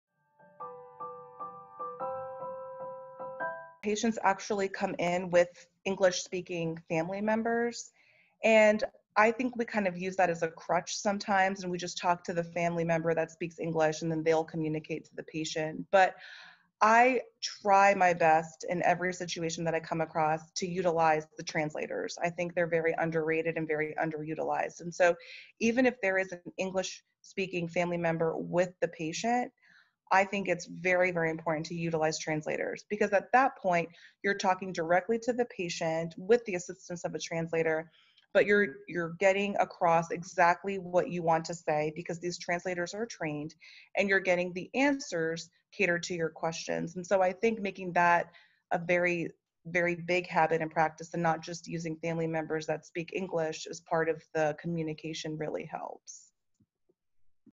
Dr. Komal Parikh: Overcoming Language Barriers
Dr. Komal Parikh discusses how to overcome language barriers.

for cultural health and health disparities insights.

Follow and like us today at